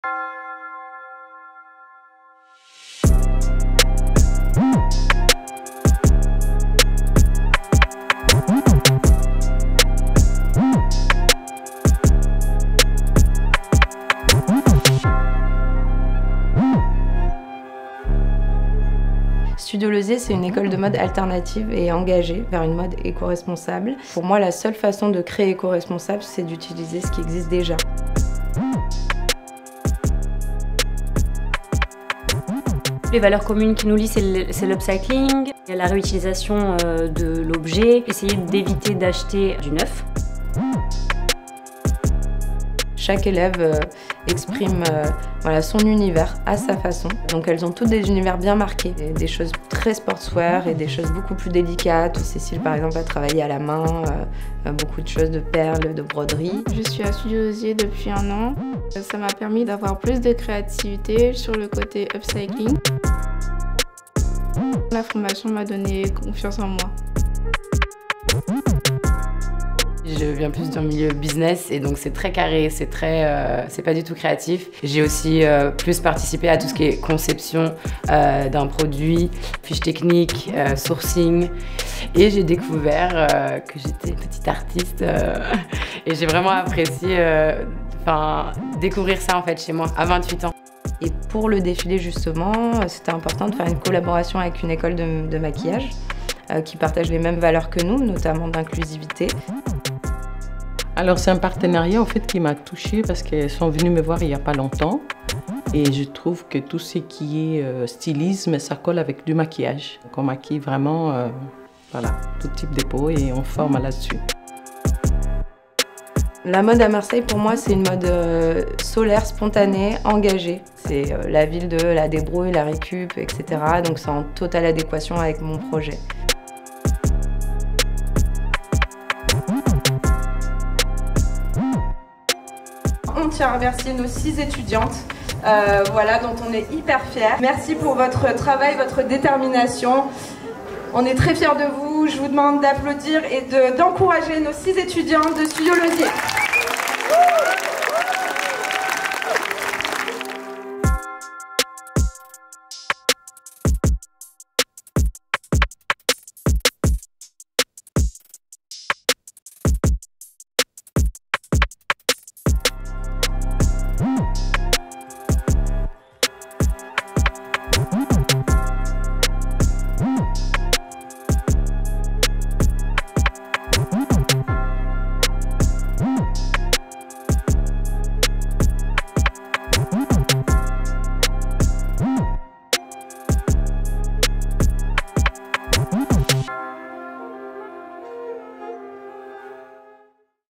Huitième Nord - Studio Lausié x Kourtrajmars
Le mini documentaire Huitième Nord est une plongée au cœur du premier défilé de Studio Lausié, école de mode alternative et engagée implantée à Marseille depuis 2021.

Des préparatifs au jour J, on découvre les six élèves de la première promo, et l’univers de leurs collections, dévoilées à Buropolis lors d’un événement en plein air inoubliable le 21 Mai 2022.

Le film a été réalisé par Mélaine Coleiro (@hella.ya.r), et produit et monté par les bénéficiaires du chantier d’insertion de Kourtrajmé Marseille. Le film a été projeté au cinéma des Variétés le 7 novembre 2022.

Ce projet est un exemple de nos collaborations audiovisuelles avec des associations et des projets pédagogiques locaux qui partagent nos valeurs.

Étudiants
Réalisatrice: Mélaine Coleiro
Chargée de production: Aminata Diaby
Cadreur: Otmane Idrissi
Cadreur: Mohamed-Ali Ahmaidi
Cadreur: Damien Chaumery
Perchiste: Grégory Vivant
Monteuse: Marine Frechon
Monteur: Gaël Barboza

Encadrants Technique
Production: Anaïs Bremond
Production: Mouezi Fatoumiya
Réalisation: Marie Vinay
Image: Mickey Aubertin
Son: Sébastien Martinez
Montage/Étalonnage/Mixage Son: Alexis Sandoval

➡️  Suivez-nous !